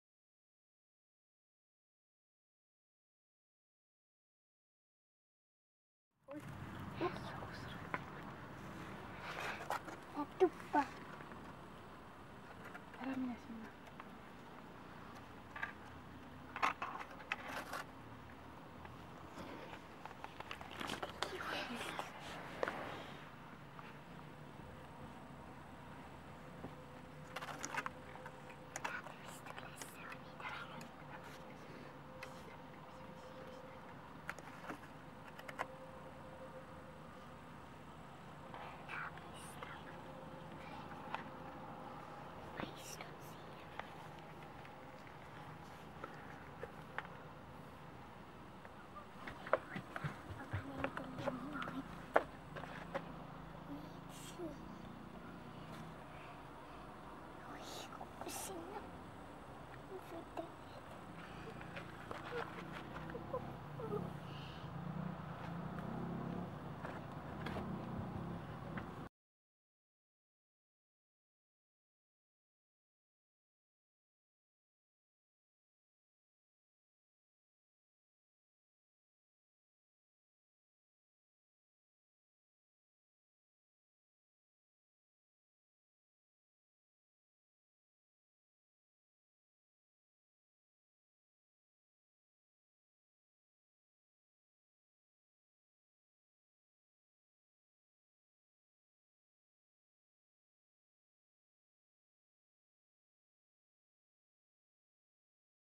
Siil külas
Augustiõhtutel luusivad aedades ringi siilid. Enam pole kaua jäänud, et siil saaks ennast talveuneks korralikult energiat täis laadida.

(CC) Video: Kaido Einama, Reisijutud.com, 12. august 2012.